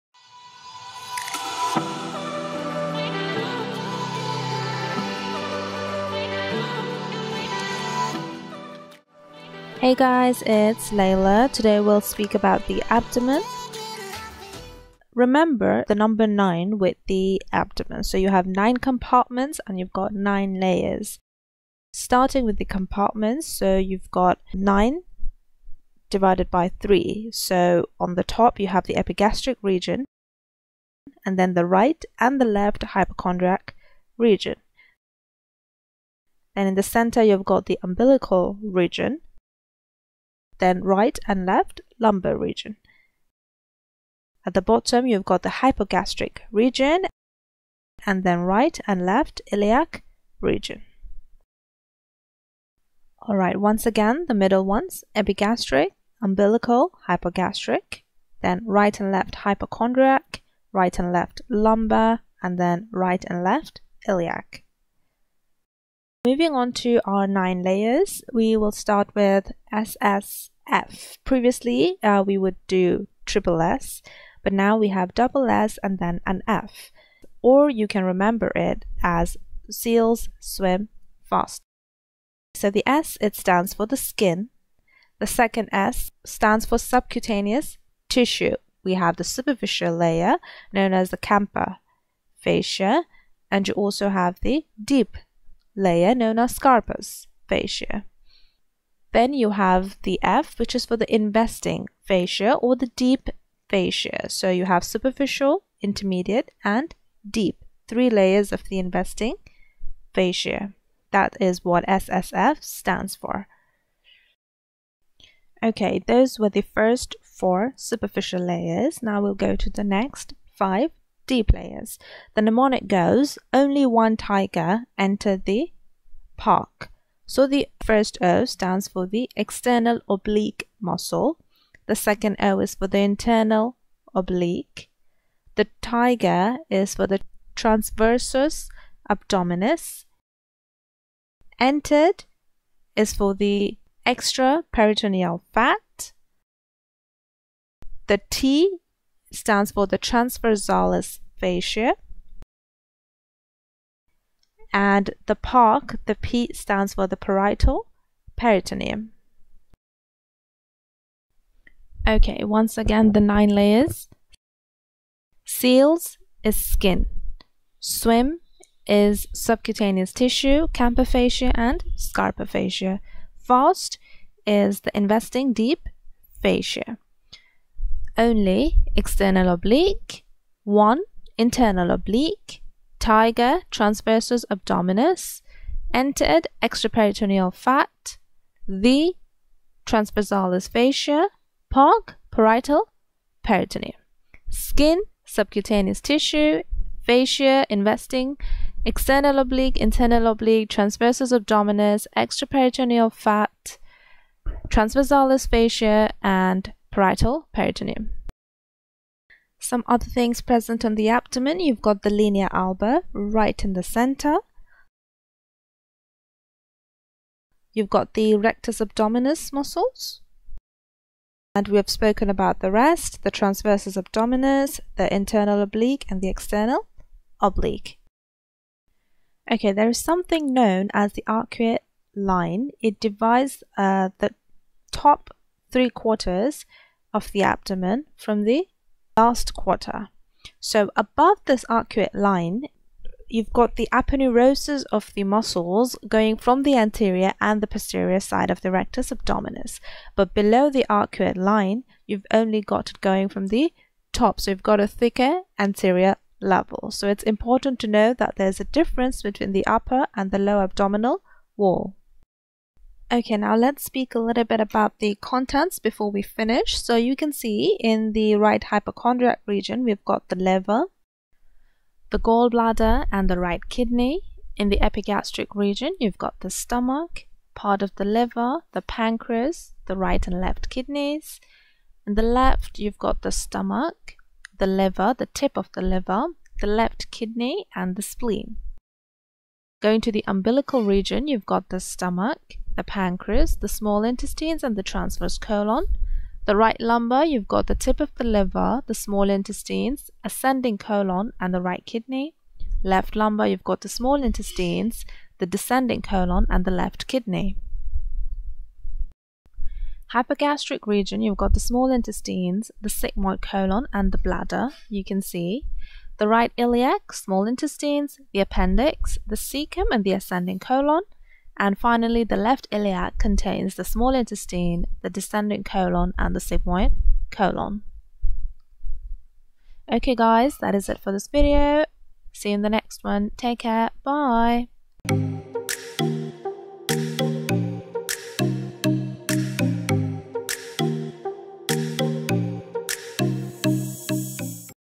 The Abdomen - Compartments & Layers
In this video I speak about the 9 compartments and layers of the abdomen, the contents and differences between upper and lower abdomen.

Mnemonic; 1) Seals Swim Fast.
                     2) Only One Tiger Entered The Park.